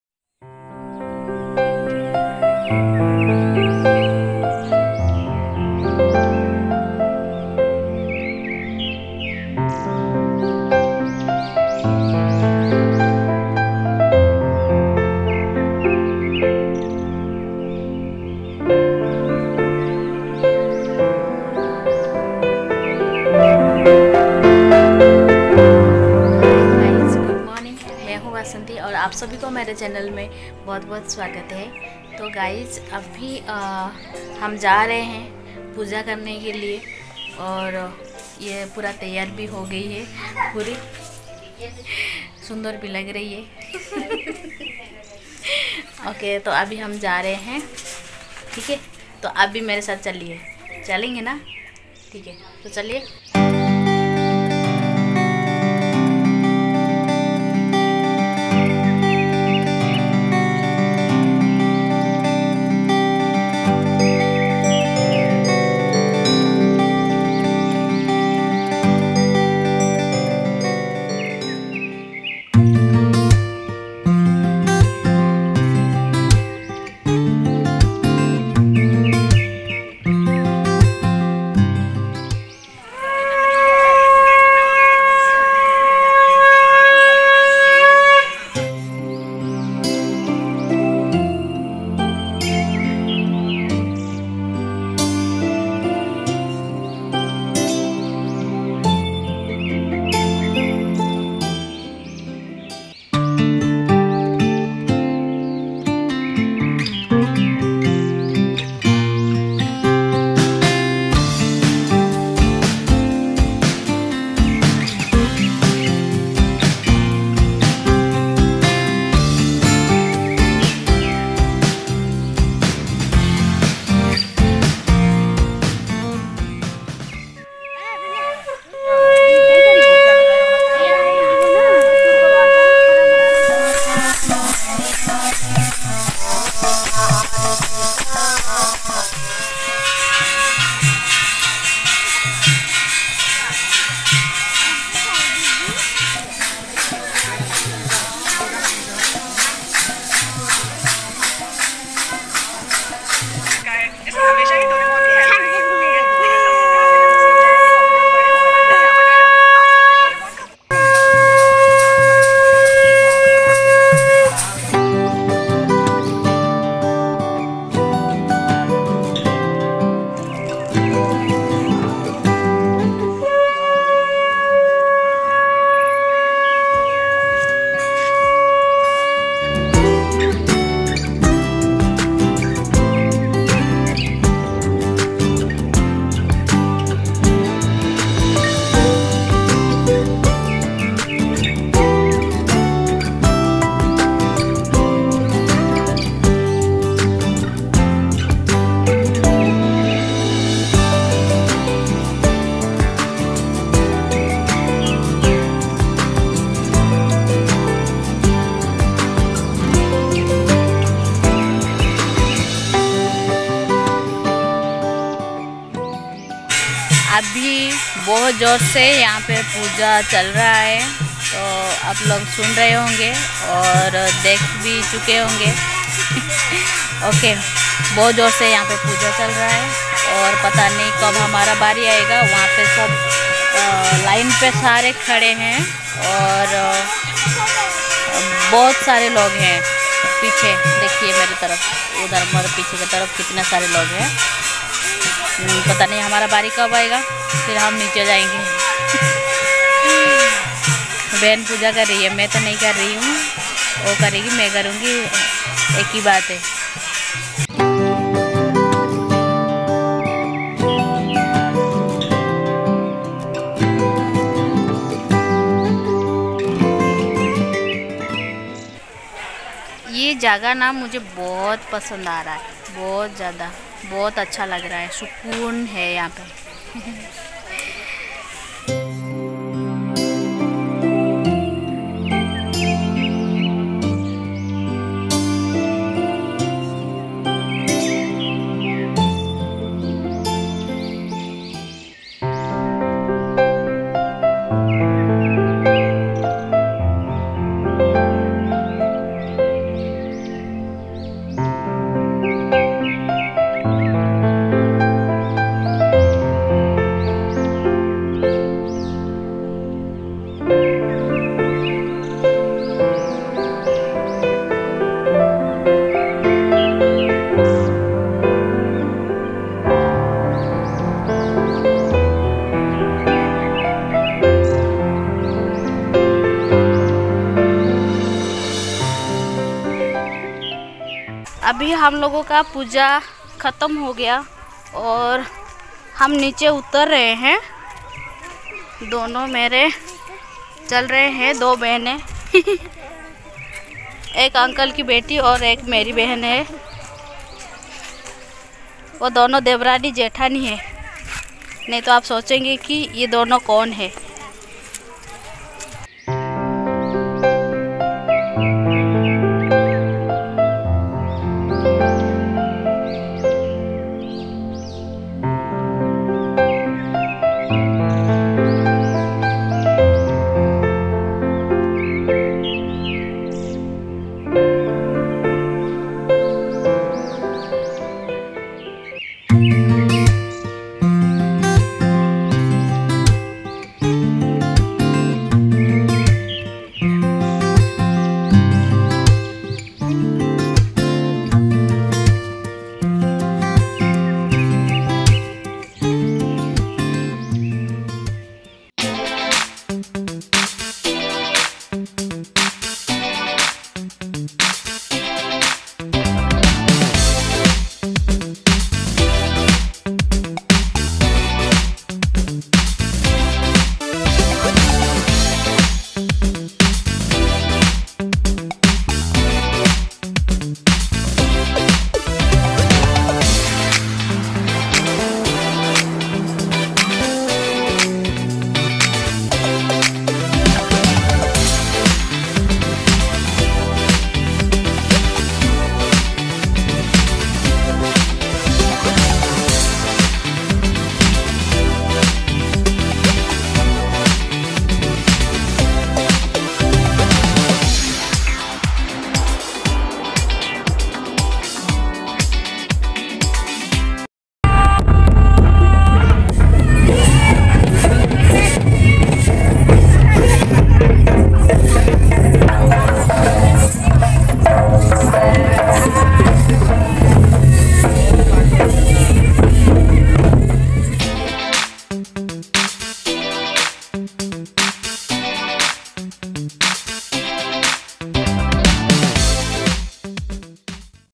Bana Devi Puja || in Holi Mahottsav || Mayurbhanj....Part -1.....❤️❤️
Aaj ham holi main bana devi ka puja karenge aur uttsav manayenge. Ummid karti hun ki aapko ye video bahat pasand ayega.

keep me support
Miss India XYZ

Some of my others videos:-